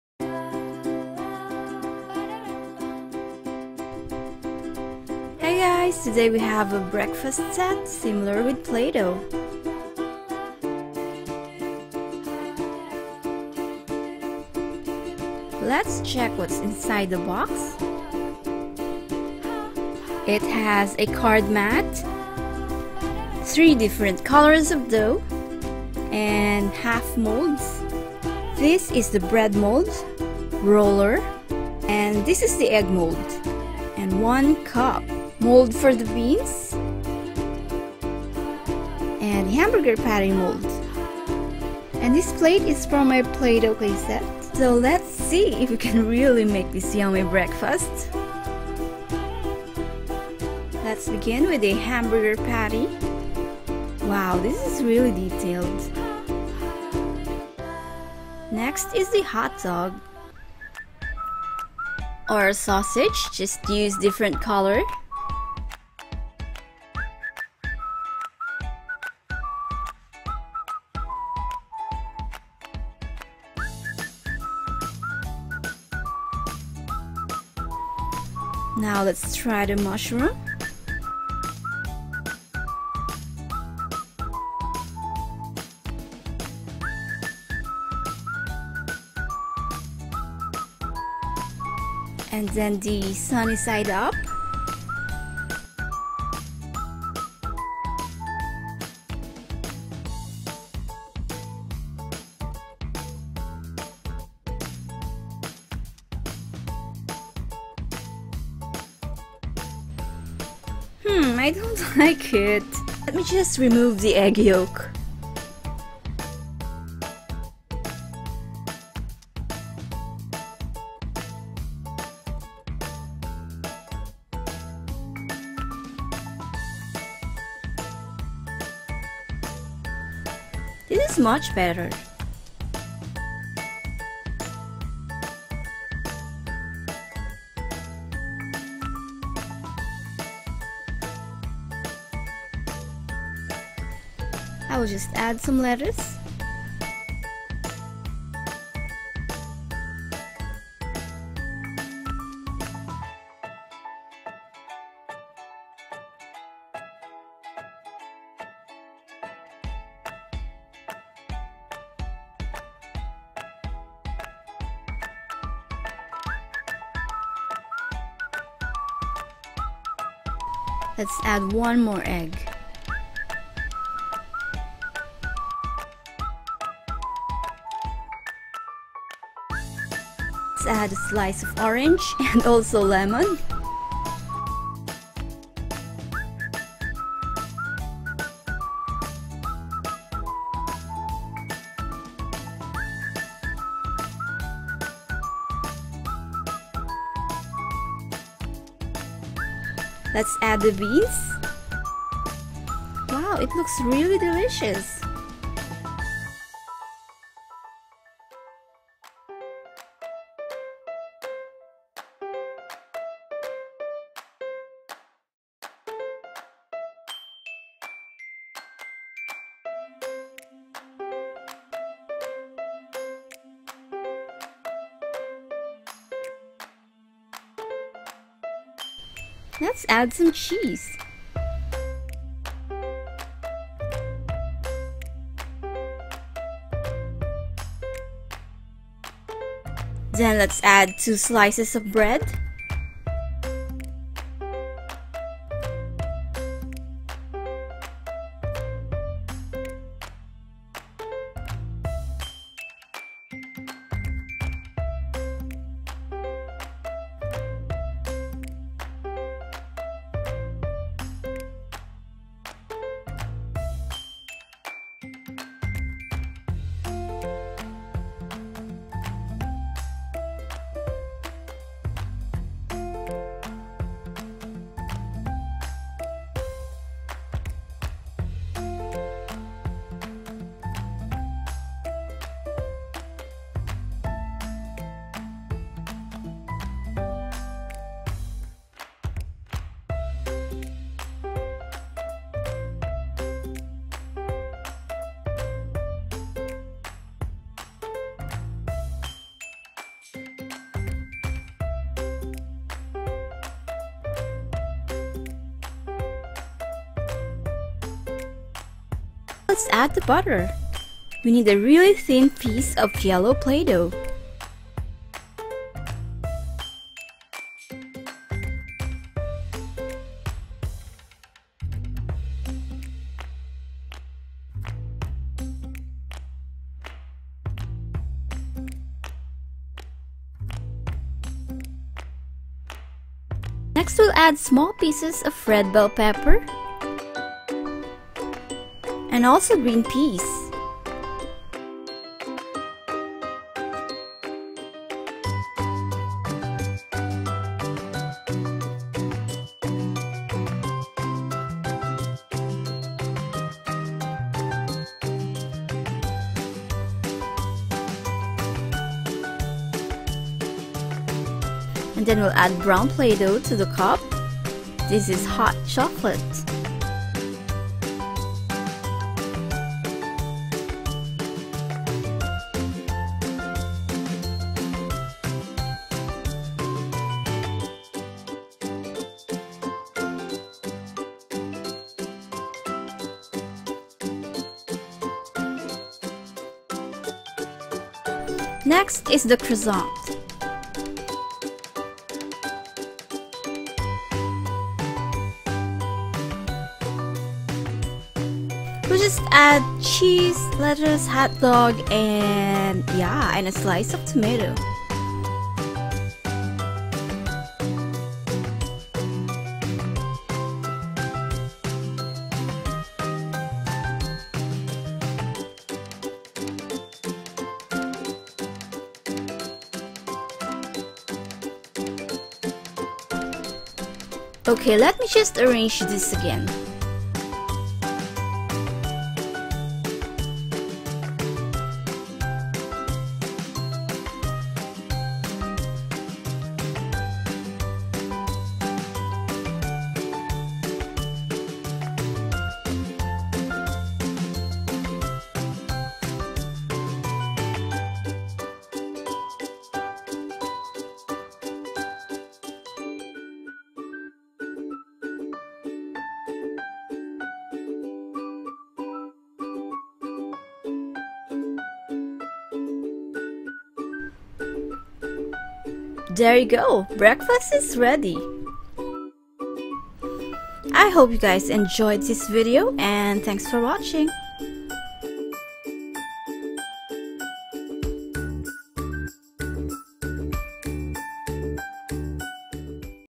Breakfast set like Play doh / burger hotdog mushroom sandwich playdoh
Play doh breakfast set made from. It has different kinds of molds for breakfast like bread, egg, beans, tomato, cheese hotdog, sausage. Super yummy breakfast dough set.

In this video, I only used Play doh.